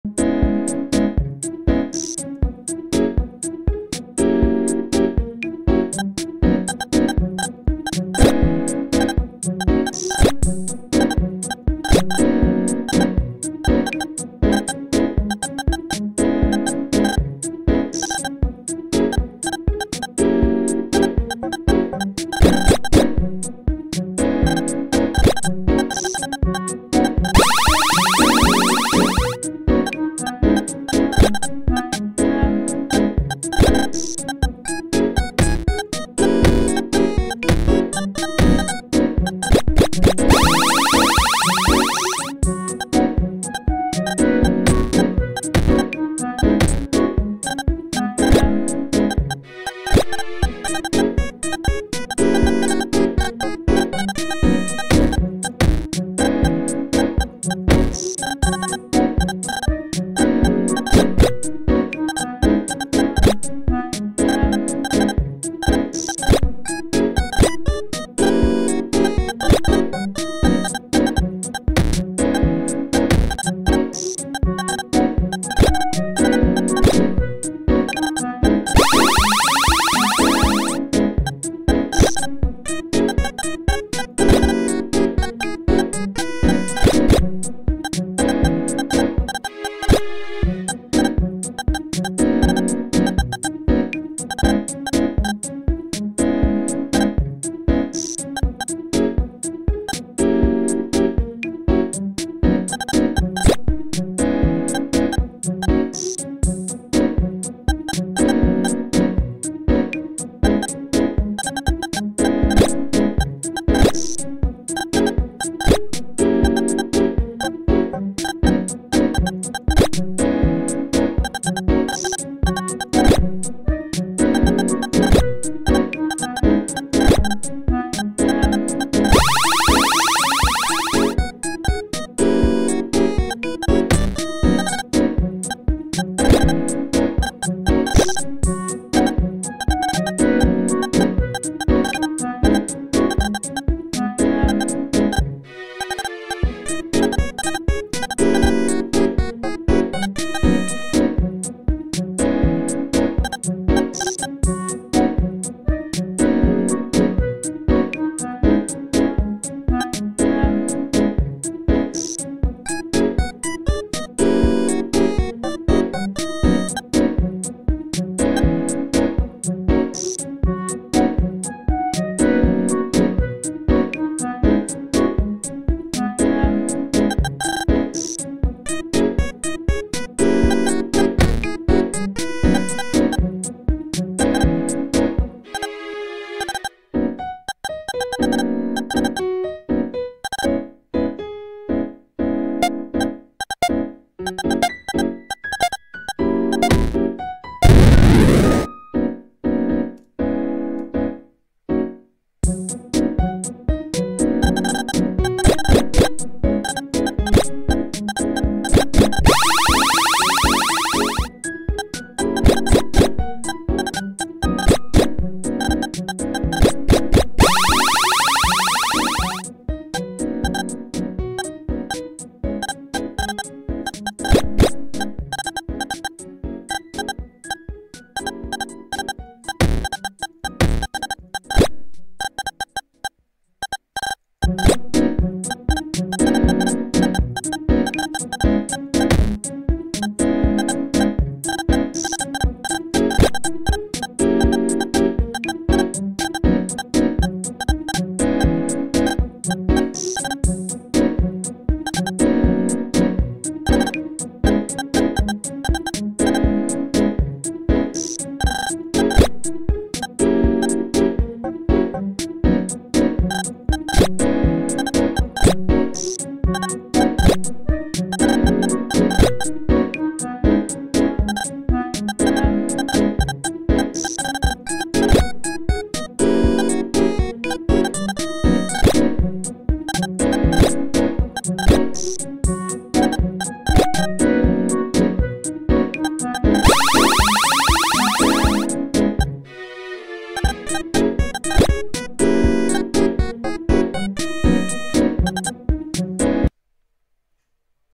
CS50 Match 3(Lecture 3) Assignment
In this Video, I will demonstrate the required features of the assignment Match 3.

00:00 Matching
00:22 Shiny Tile destroying column/row
00:45 Only swap when it results in a Match
01:53 Board reset if there are no possible Matches